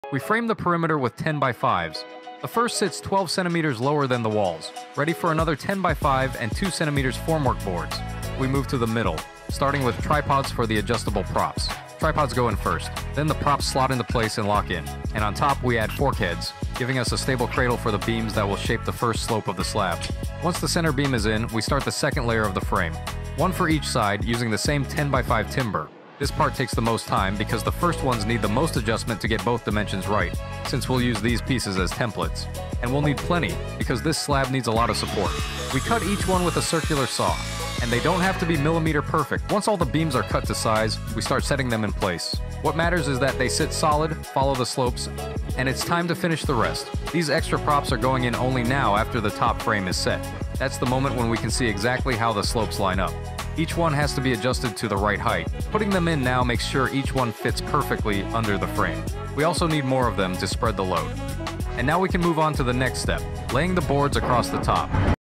Before the Pour… Comes the Frame 🧠 | Concrete Slab Formwork
Before any concrete hits the site, the slab needs a solid frame.
Here’s how the formwork takes shape — the backbone of every strong slab.